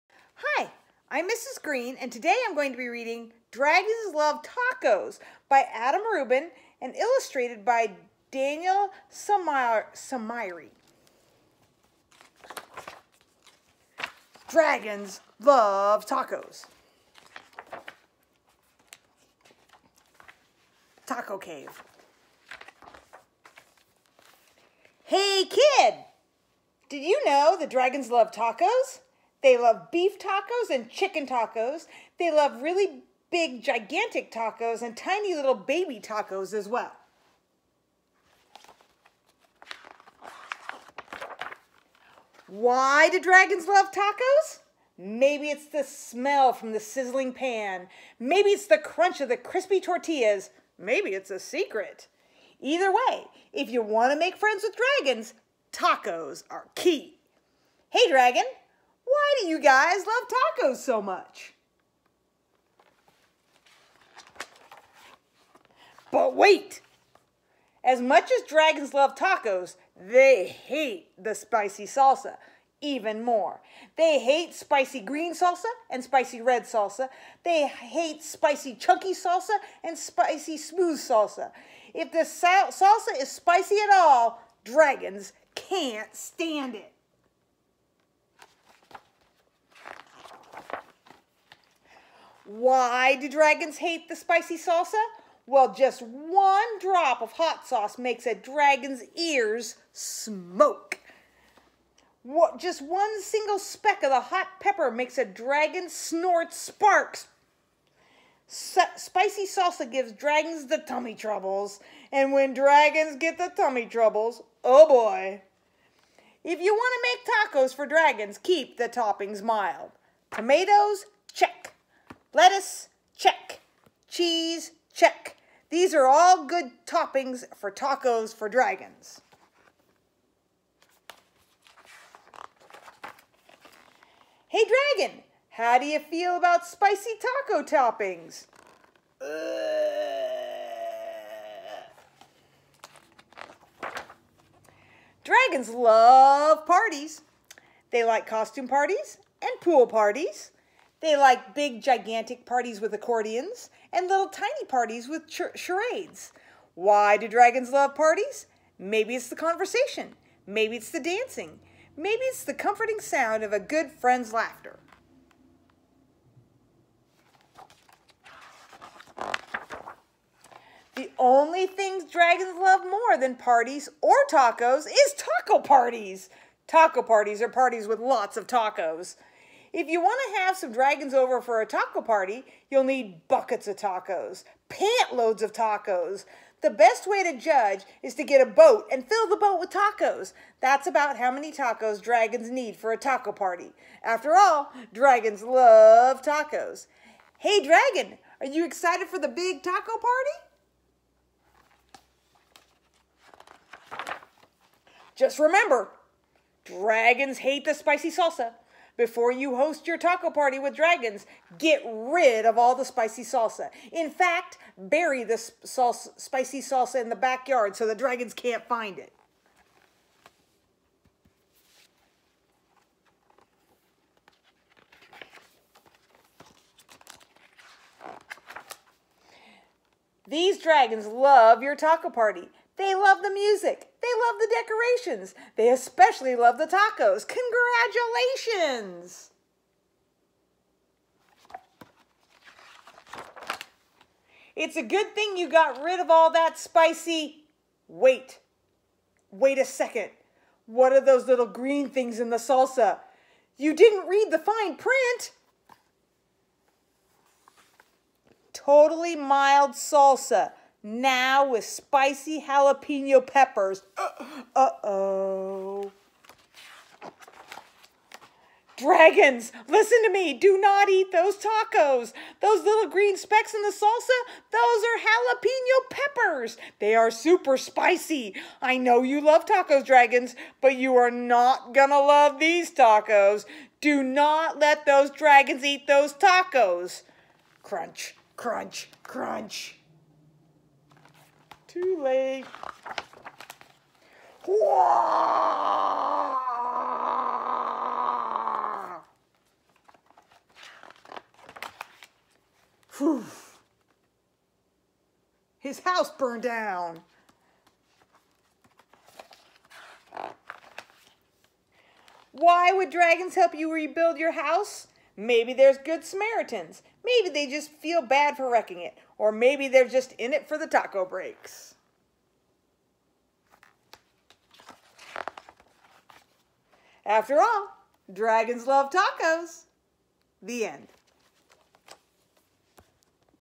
Mrs. Green reads Dragons Love Tacos
Dragons love tacos.  Doesn't the title just make you want to read the book?